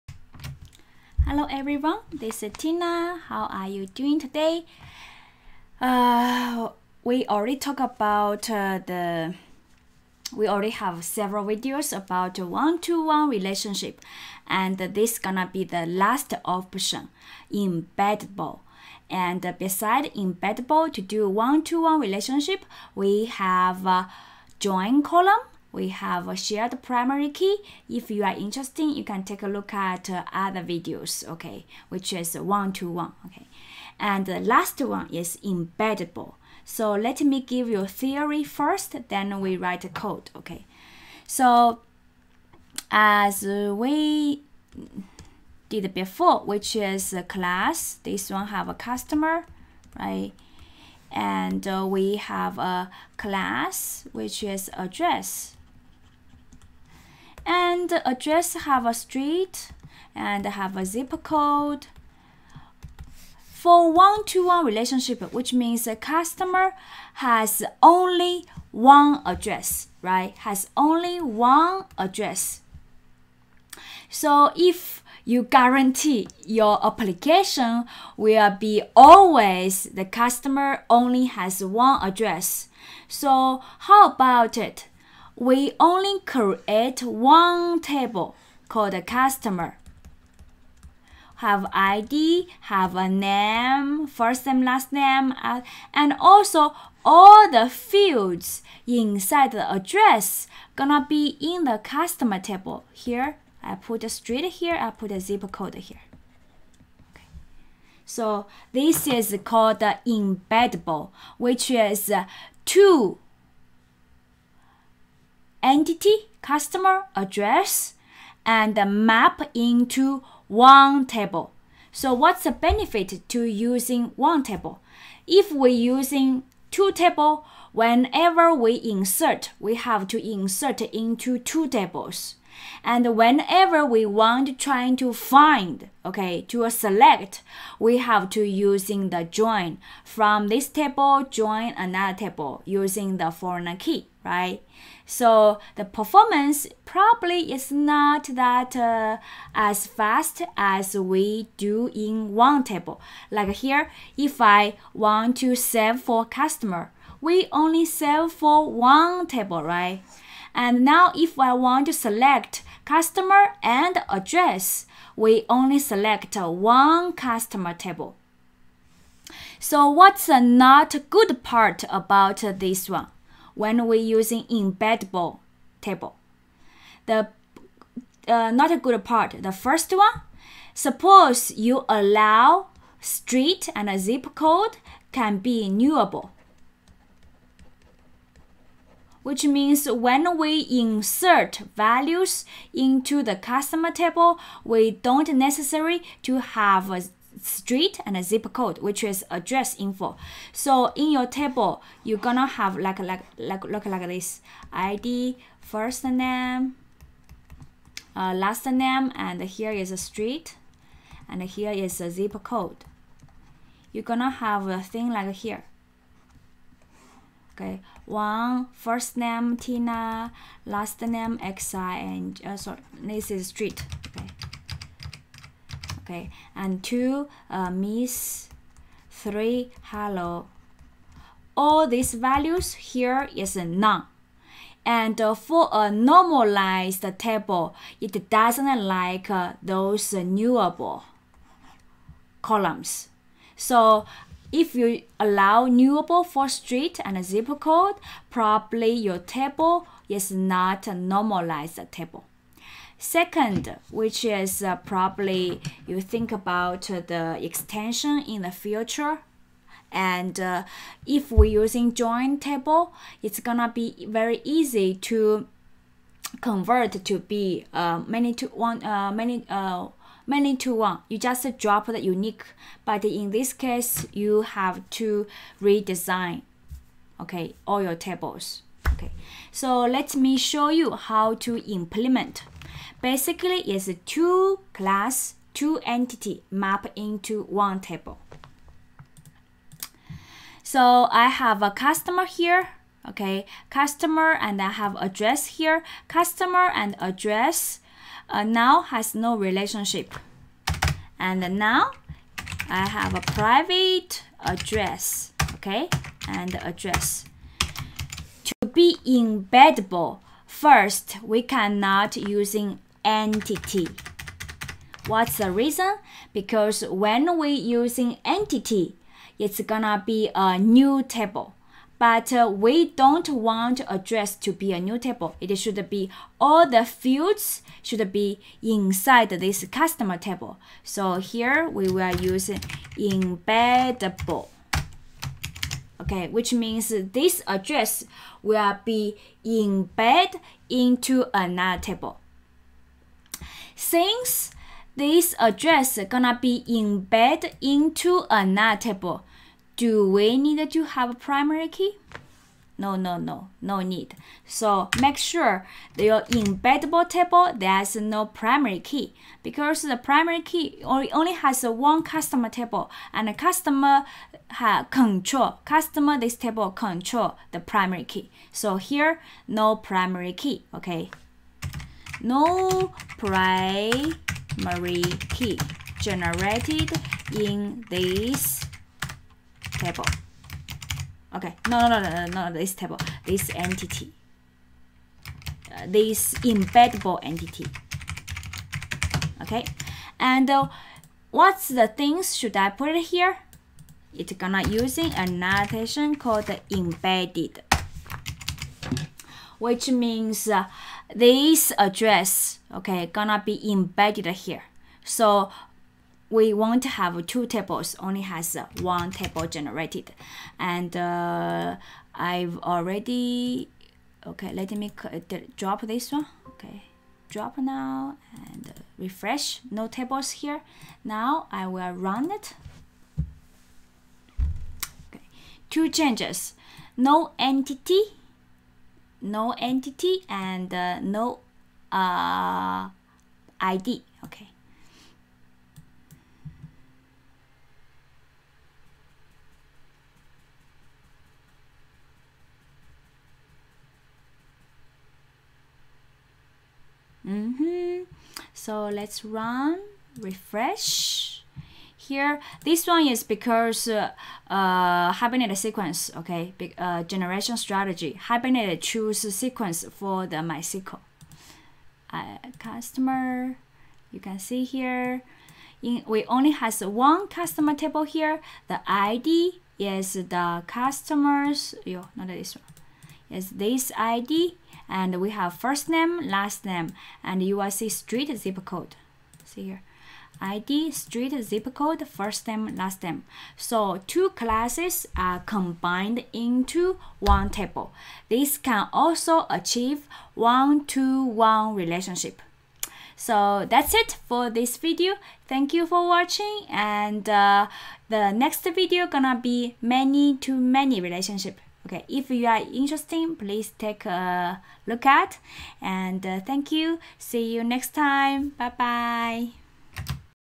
Association Mapping - One-To-One Embeddable
In this video, discuss the last option to do One-To-One Relationship which is embedding all fields from one entity to another entity.